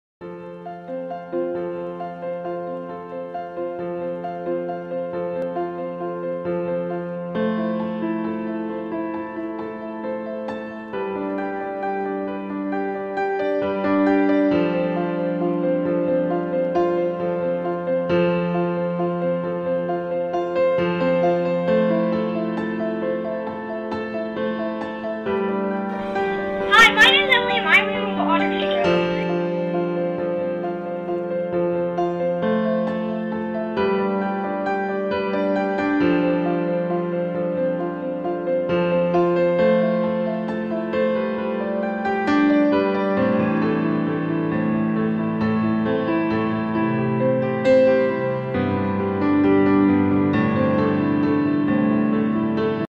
Emily's Archimedes Water Screw project
In 2009, Emily made this as a school project.